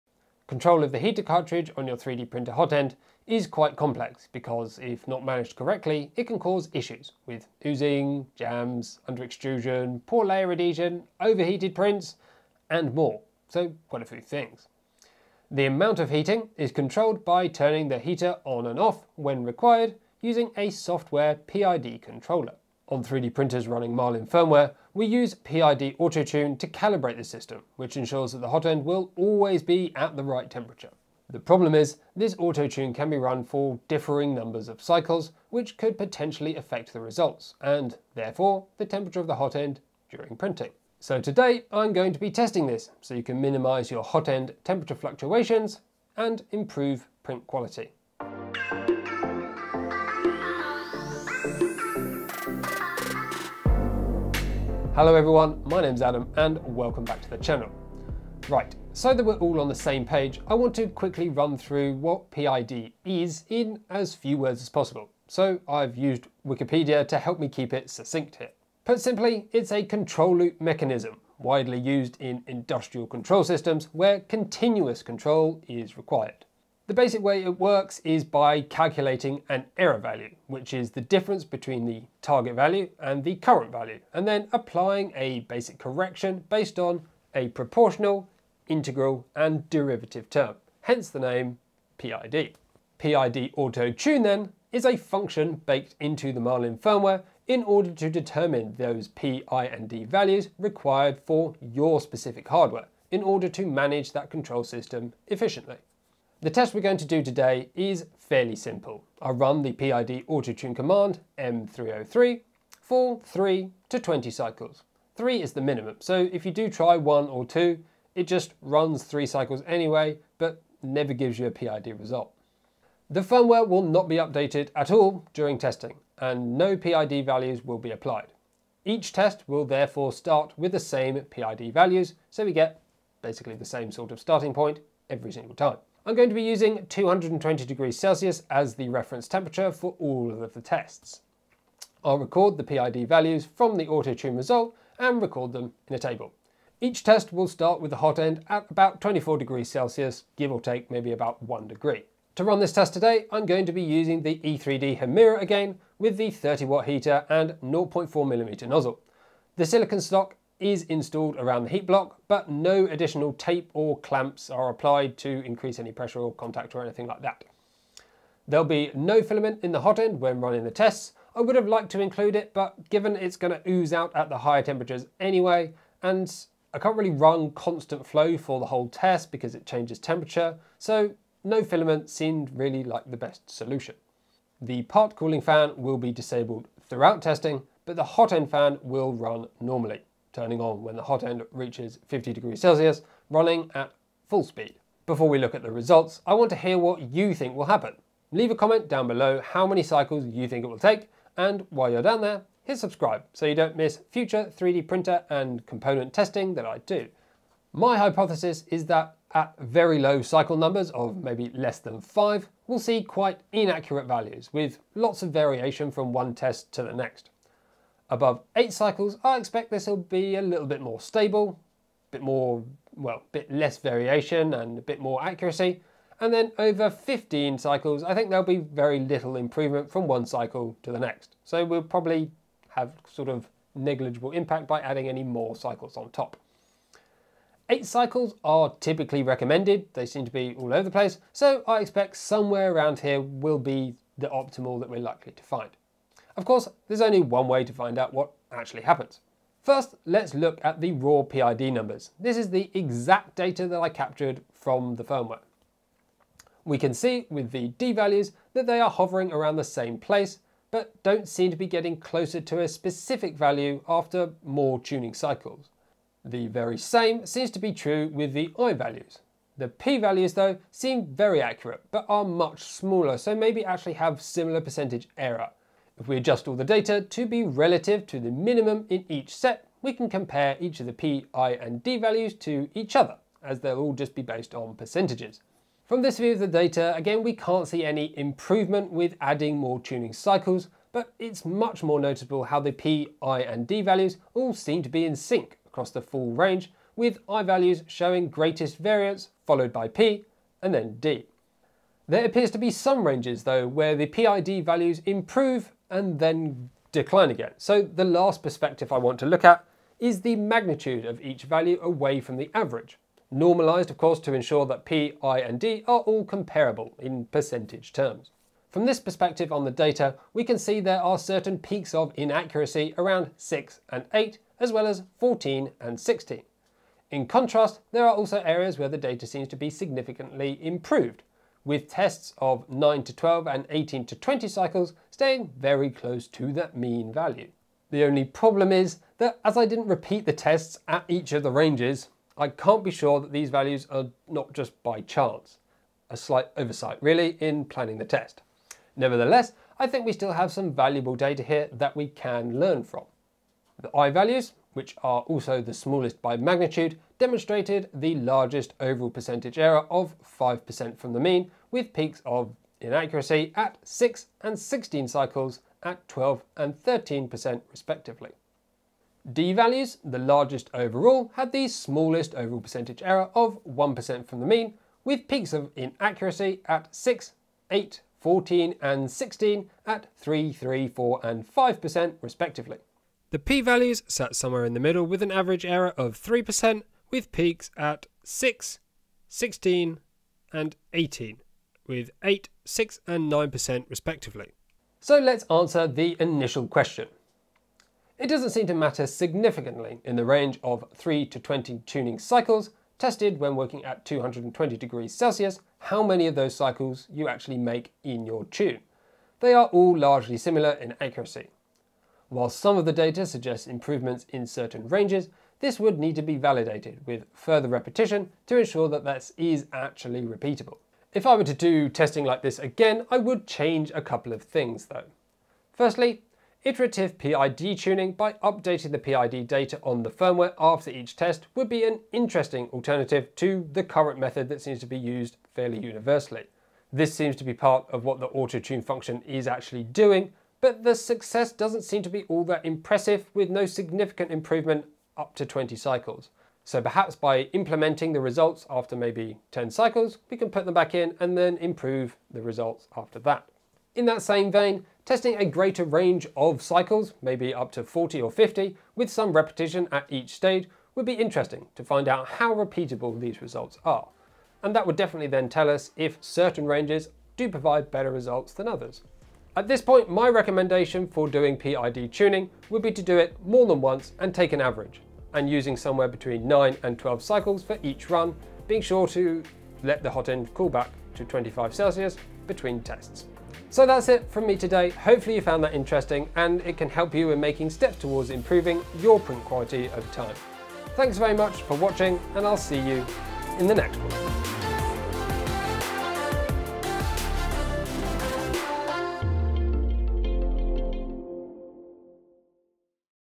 This is NOT what I expected....
As one of the details that could affect hot end flow rates, PID parameters are important for high and consistent printing performance. In this episode we take a detailed look at the autotune feature and how many cycles you need to tune your hotend.

0:00 Introduction
0:54 PID Explained
1:45 PID Testing Plan
2:32 Test Apparatus
3:14 Predictions
4:06 RAW PID Test Data
4:35 Relative PID Data
5:06 Magnitude from the mean
5:52 Summary of Data Capture
6:34 Conclusions
7:01 Lessons learned
7:49 Recommendations
8:03 Ending

Since finishing this video I think powering off the printer and starting fresh the next day may have had a significant impact on the results. There seems to be some data carrying over to the next test.

Tools
Wera Hex Keys
Wera Ratchet and Socket Set:https
Knipex Cutter
Ifixit Tool kit
Wire Stripper Tool
3M Applicator Tool